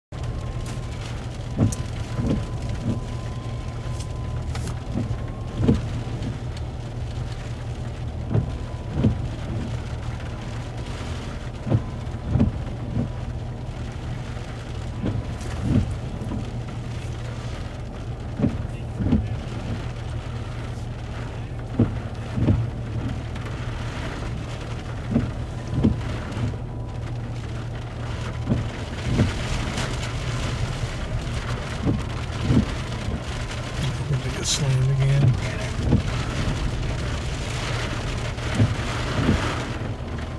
Tornado Warning SW of Seminole, Oklahoma
Chased this storm from Rush Springs to Seminole, Oklahoma. Had a persistent wall cloud but no tornado.